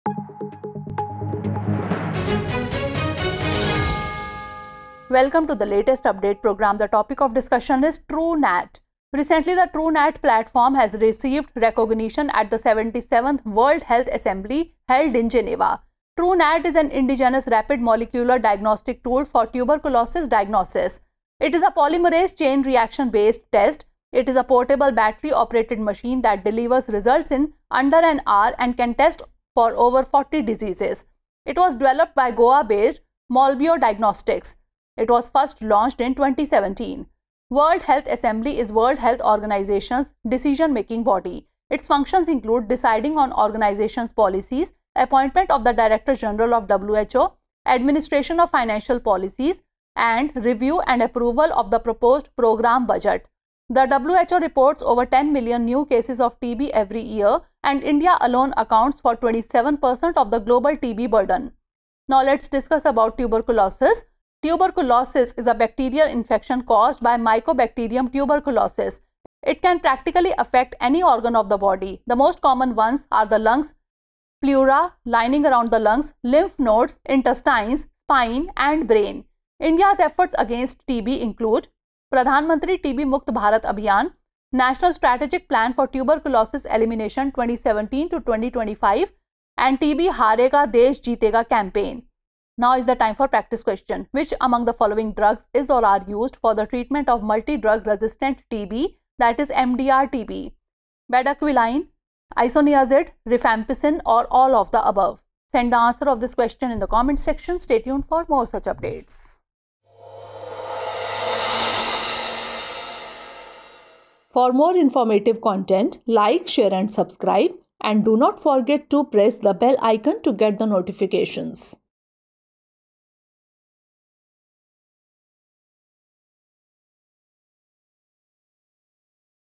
Truenat | 77th World Health Assembly | Latest Update | Drishti IAS English
The Truenat platform, an indigenous rapid molecular diagnostic tool for tuberculosis (TB) diagnosis, has recently gained recognition at the 77th World Health Assembly (WHA) in Geneva. Truenat is a portable, battery-operated machine that delivers results in under an hour and can test for over 40 diseases. It was developed by Goa-based Molbio Diagnostics. This polymerase chain reaction (PCR)-based test was acknowledged for its significant impact on TB diagnosis.